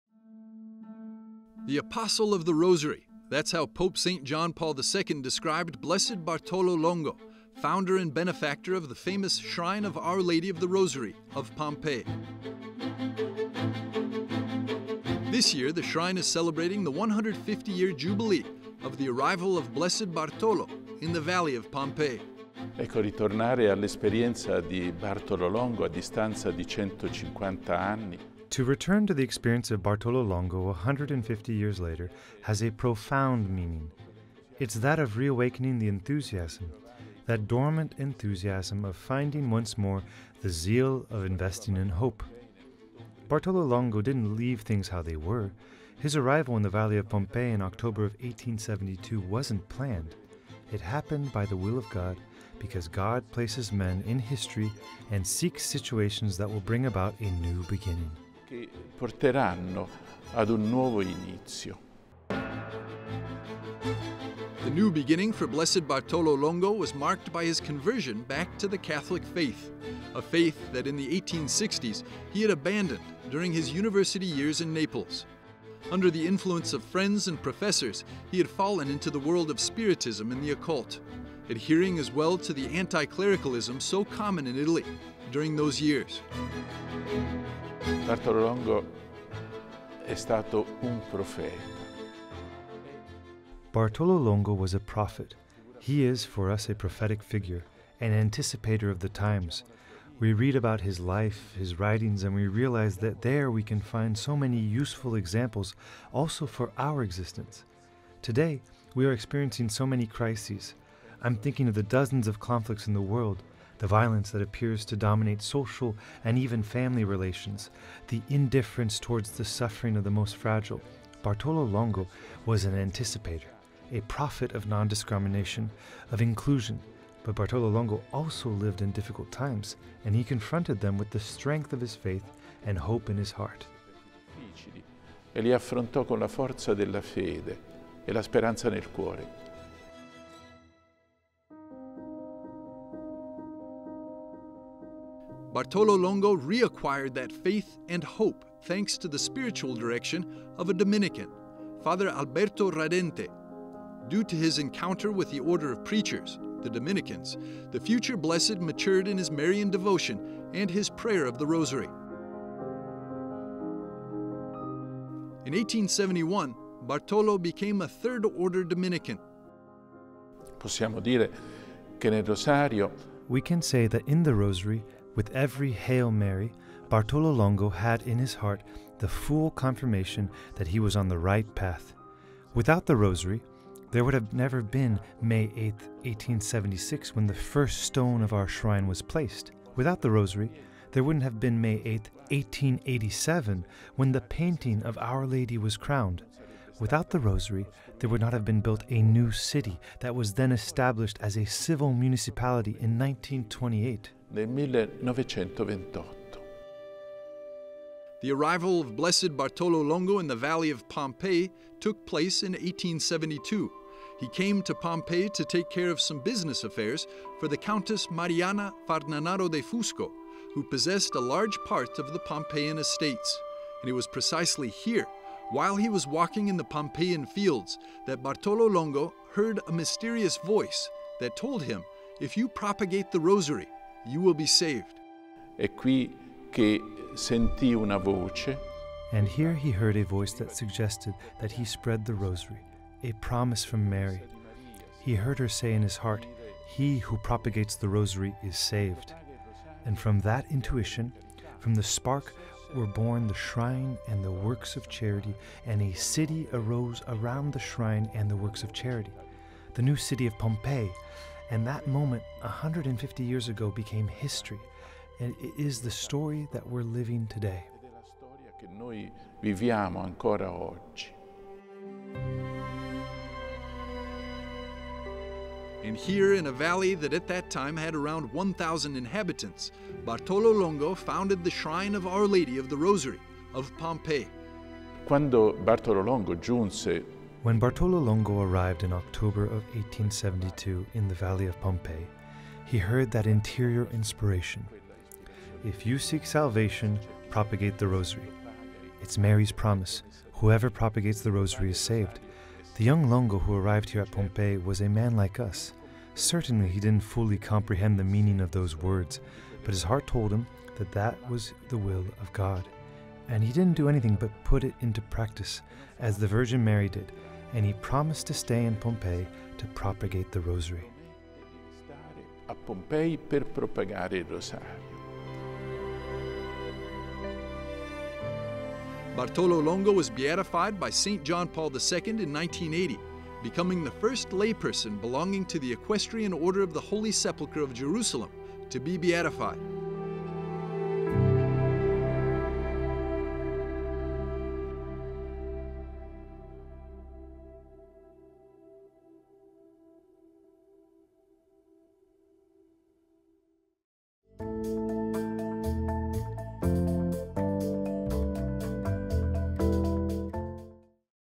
The Apostle of the Rosary: Celebrating 150 Years of Blessed Bartolo Longo, the Ex-Satan Priest
Join us in celebrating the 150-year jubilee of the arrival of Blessed Bartolo Longo, founder of the famous Shrine of Our Lady of the Rosary of Pompeii. In this video, Archbishop Tommaso Caputo, Territorial Prelate of Pompeii, shares the inspiring story of Bartolo Longo's conversion back to the Catholic faith, his devotion to the Rosary, and the founding of the shrine and works of charity that gave rise to the new city of Pompeii. Bartolo Longo was beatified by St. John Paul II in 1980, and his life serves as an example of hope and inclusion in times of crisis.